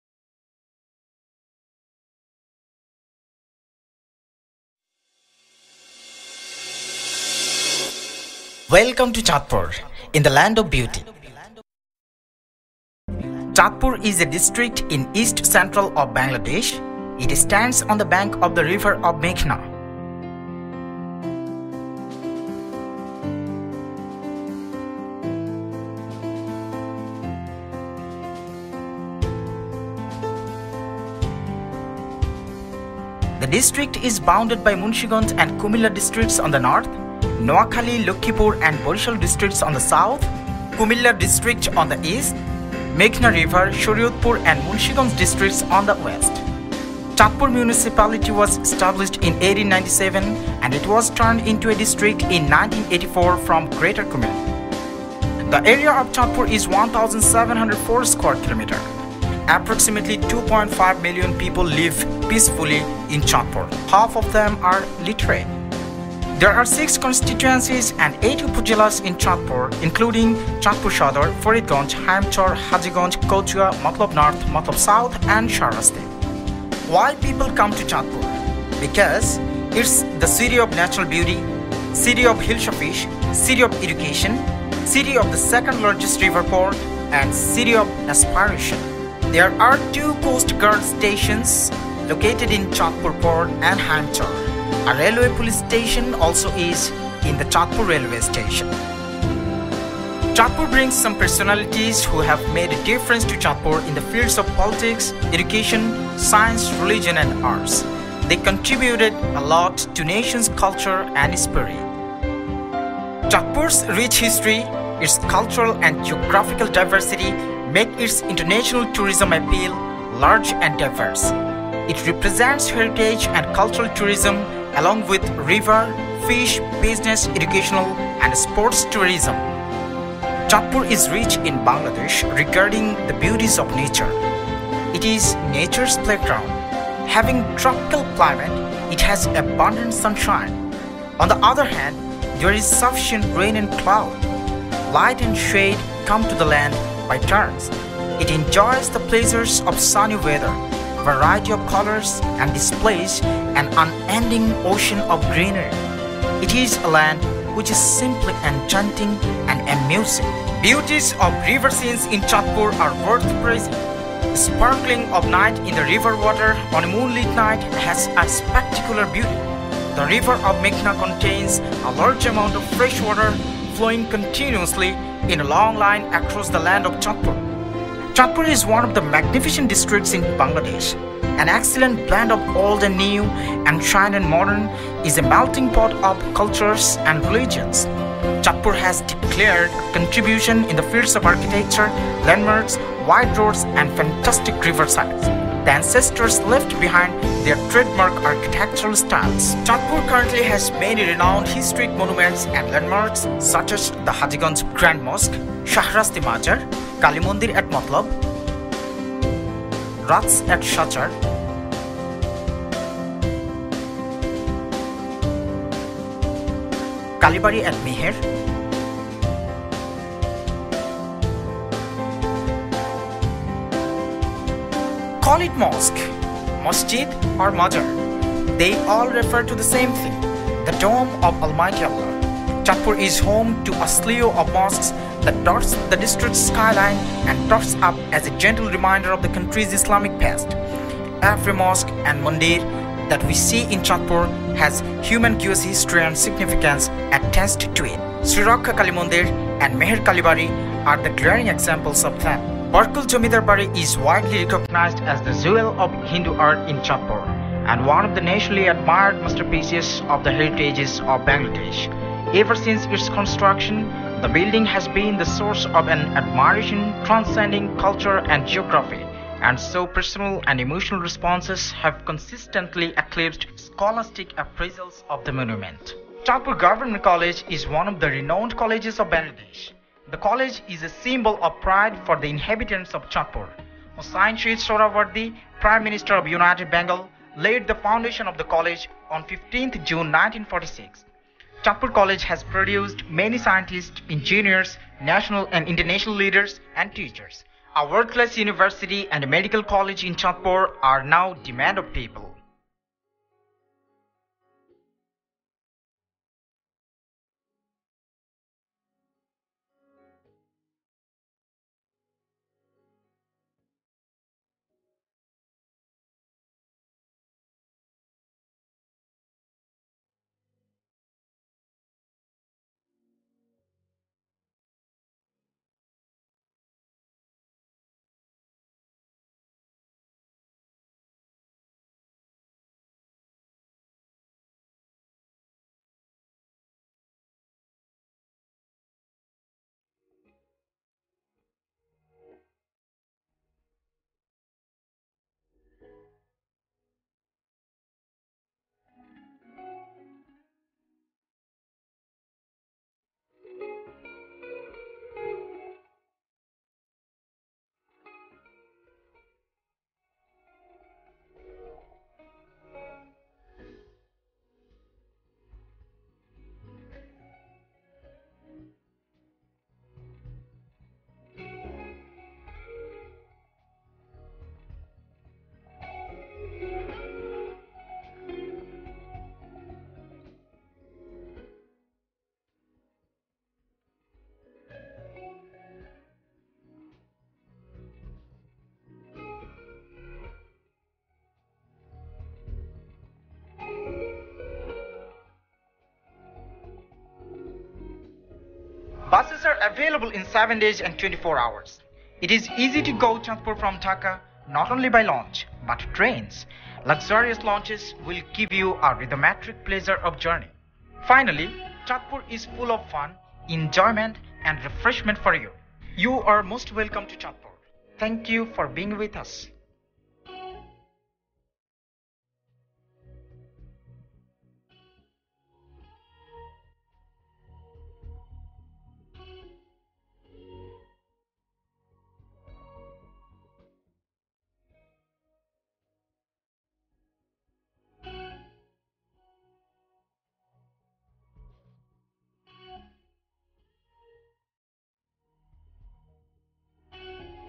চাঁদপুর জেলার প্রামান্যচিত্র -Documentary of Chandpur District
Documentary of Chandpur district in Bangladesh. Hereby you know about chandpur travels guide, chandpur beauty etc.